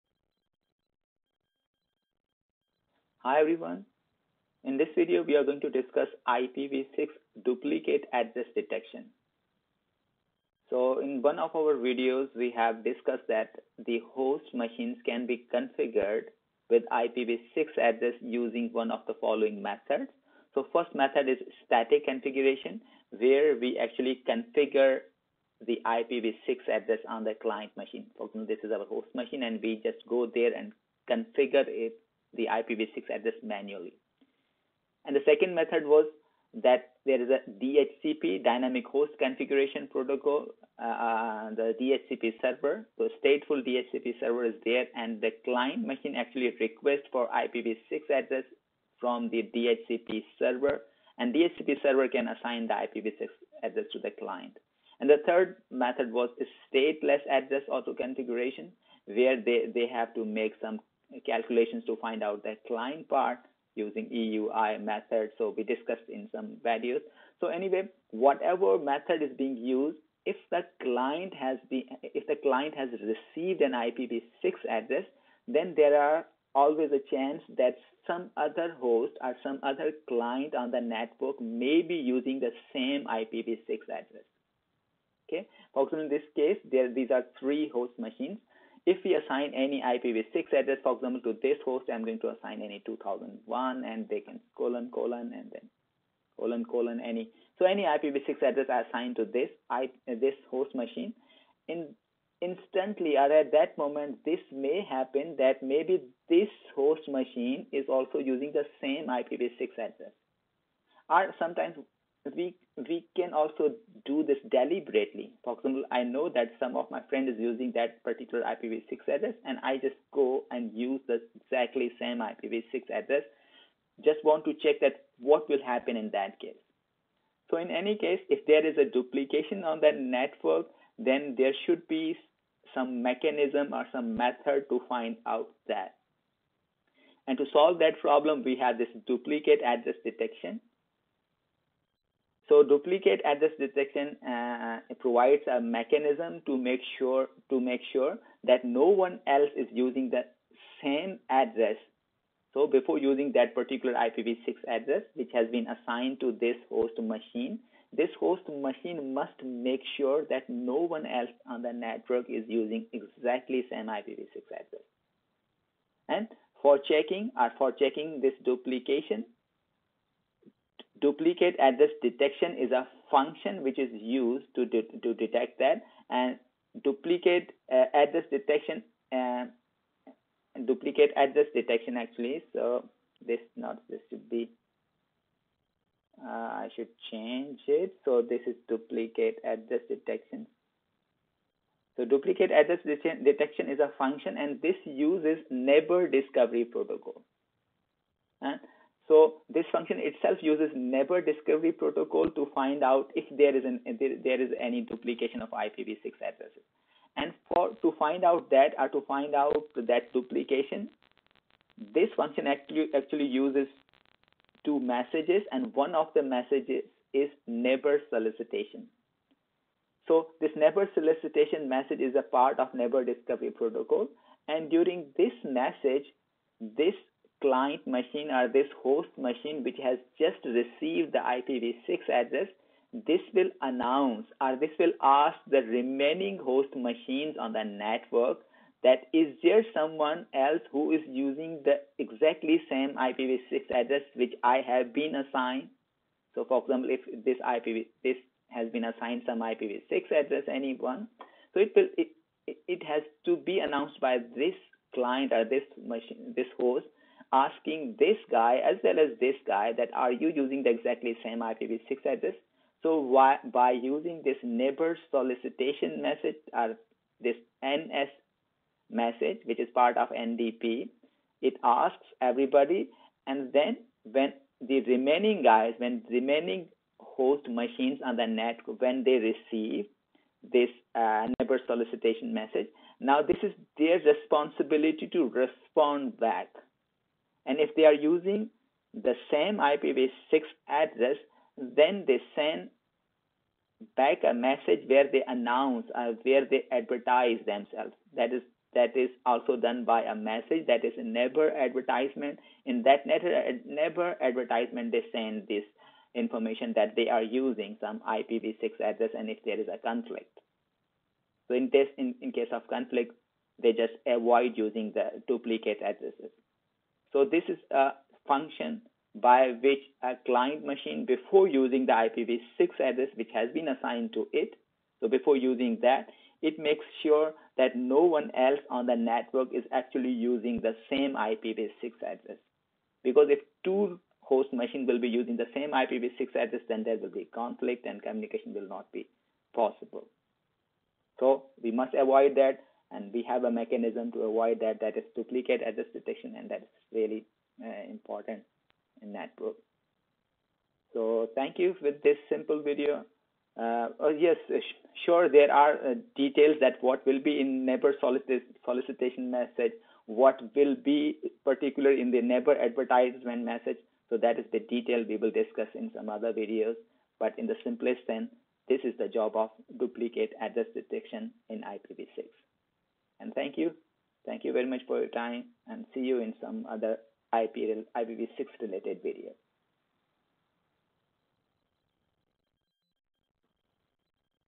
IPv6 Duplicate Address Detection | IPv6 DAD
IPv6 Duplicate Address Detection (DAD) is required to verify that only single host is using a particular IPv6 address to avoid to any conflict which may result in failure of communications.

Hosts can be configured with IPv6 address using one of the following methods
(1) Static configuration
(2)Stateful DHCP
(3)Stateless Address Auto-configuration (SLAAC)

DPD is a function using Neighbor Discovery Protocol (NDP)  to find out any conflict in using IPv6 address

Host sends Neighbor Solicitation message to all other hosts on the network asking if any of them is using the same IPv6 address.

The other host using the same IPv6 address will send back a message called Neighbor Advertisement to the host and the first host, after this message, ignores to use the same IPv6 address.